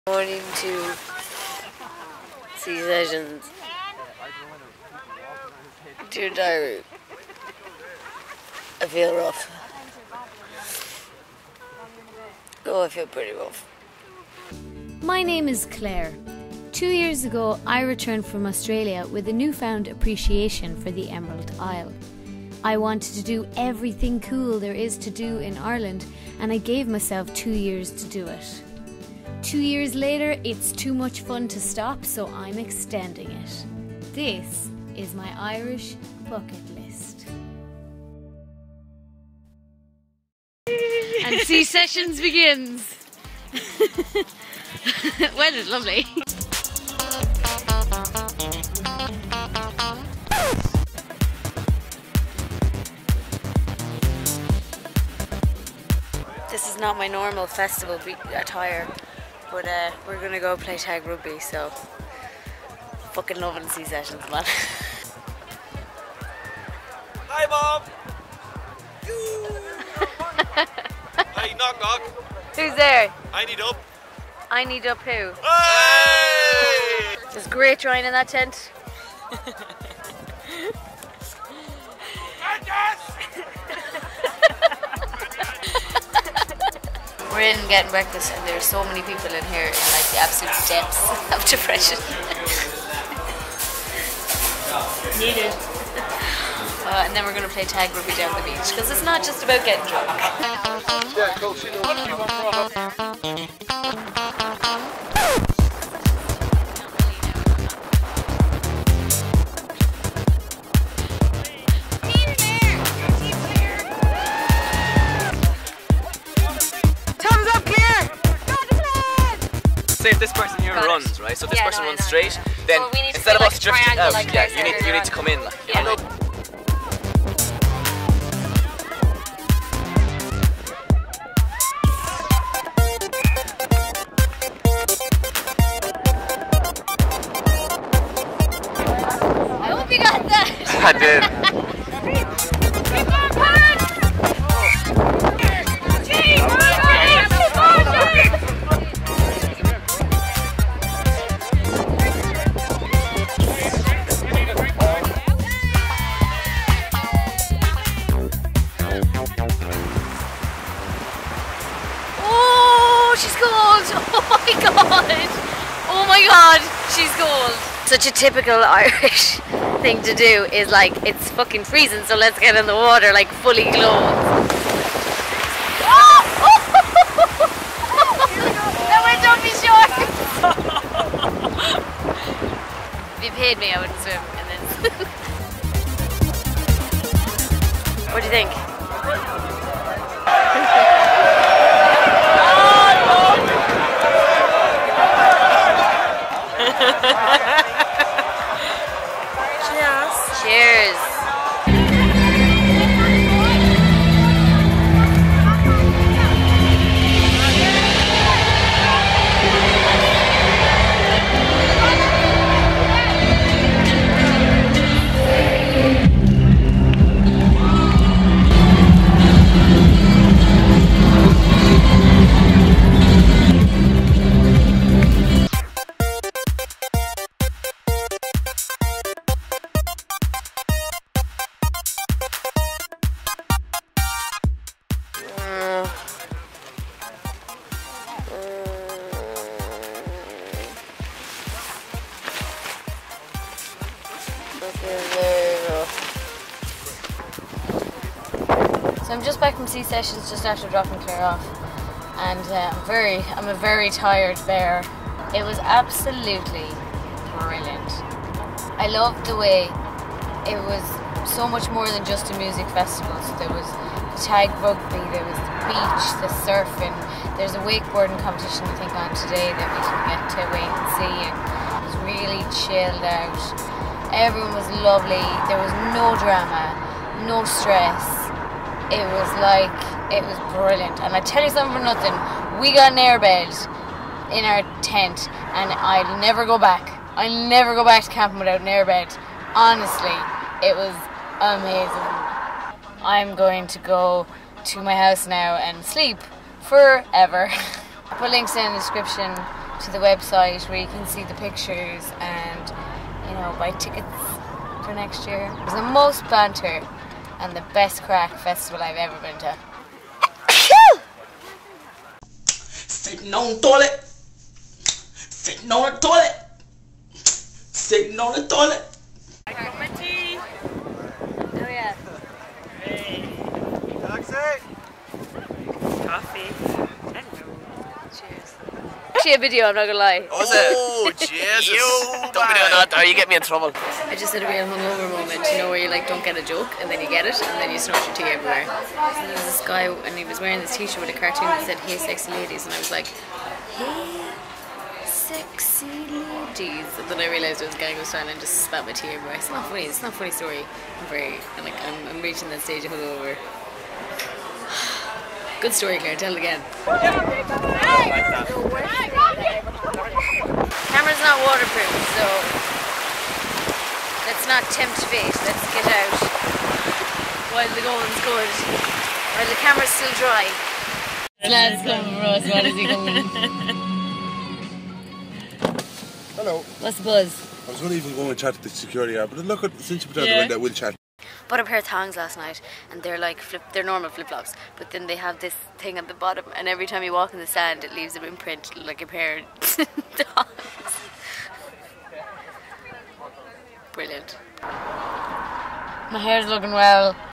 I Went To Sea Sessions Surf & Music Festival in Ireland | The Irish Bucket List (Ep #29)
We came, we played tag, we rocked out at Avicci, we didn't want to leave.

I'm trying to make the IrishBucketList into a guide book!

The equipment I use:
CAMERA: Canon 600D, Canon 60D, Nikon
MIC: Rode VideoMic Pro
EDITING: Premiere Pro